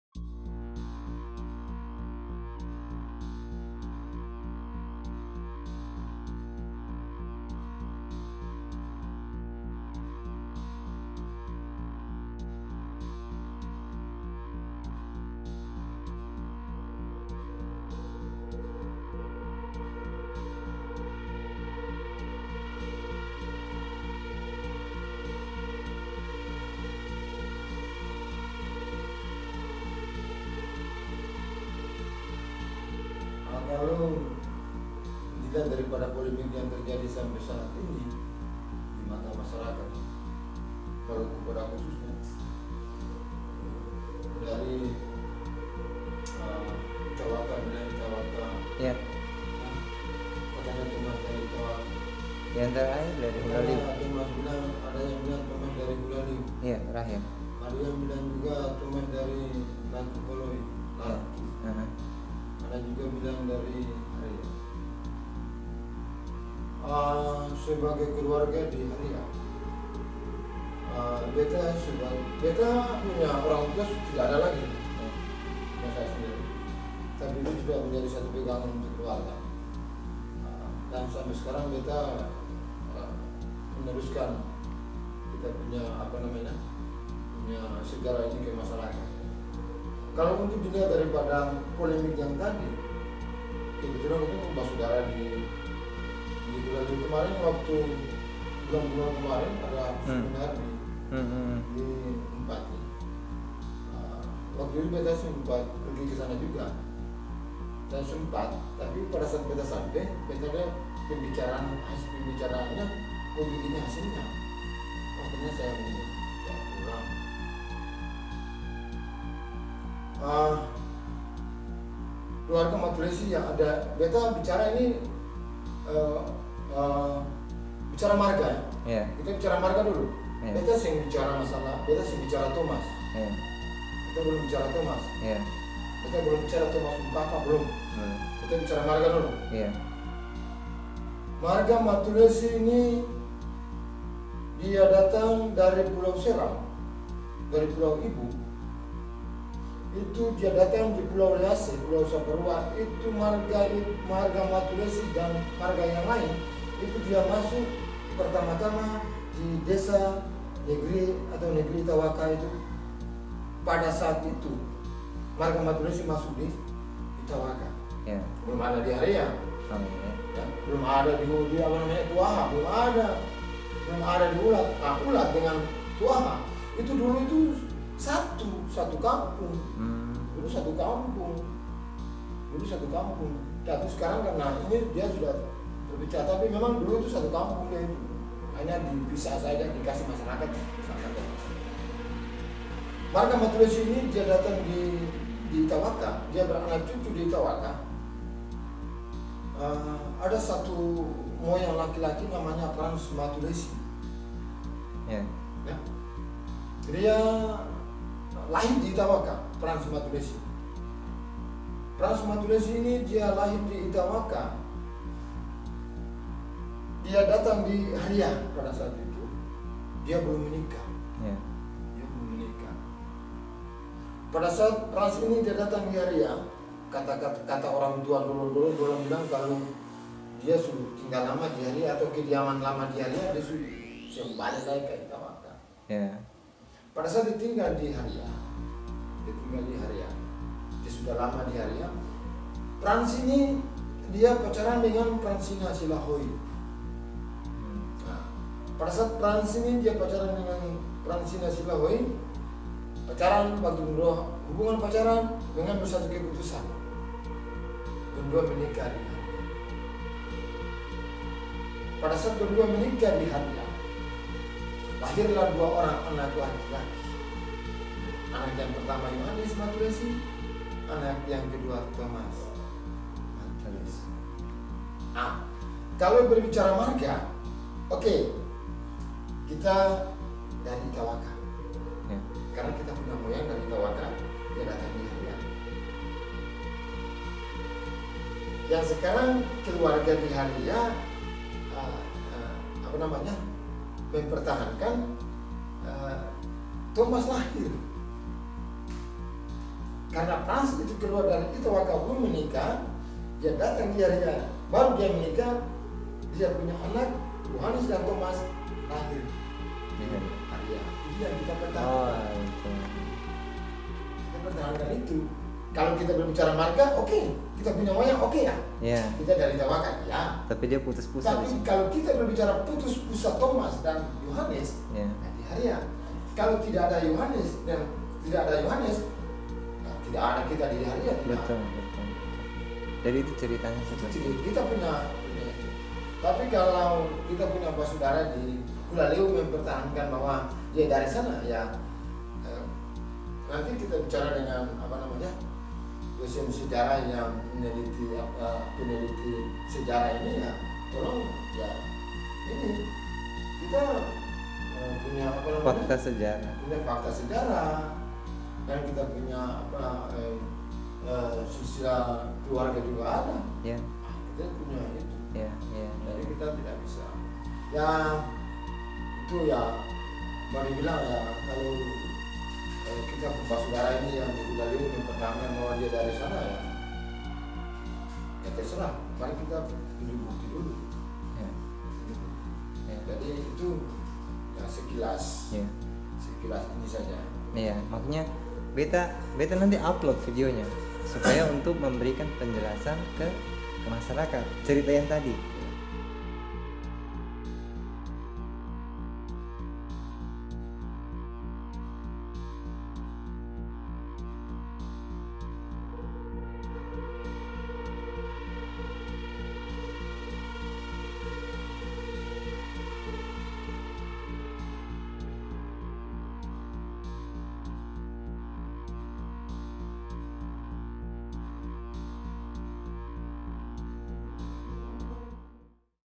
Saling Kalim Turunan Pattimura
Keterangan dari Frans Matulessy,  menjawab  kalim atas ketrunan Pattimura di Maluku yang akhir-akhir ini menghangat.